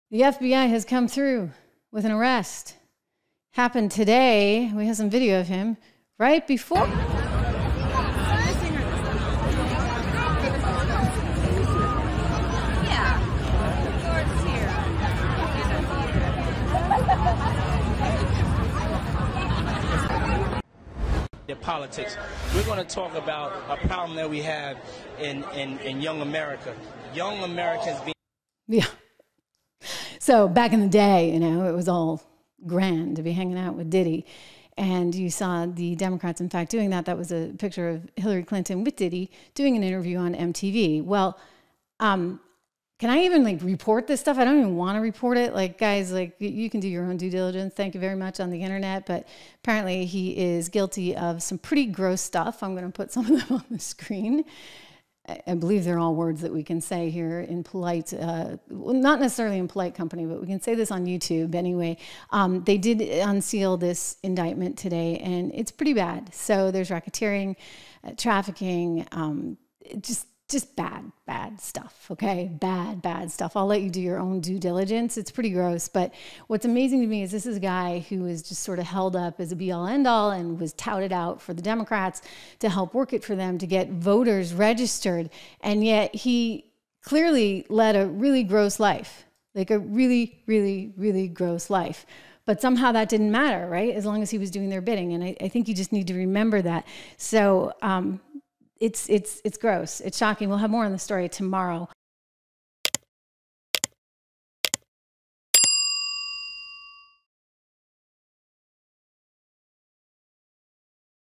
Diddy Arrested! Questions Raised as America Asks ‘Why NOW?’
The music mogul has finally been arrested— here’s why.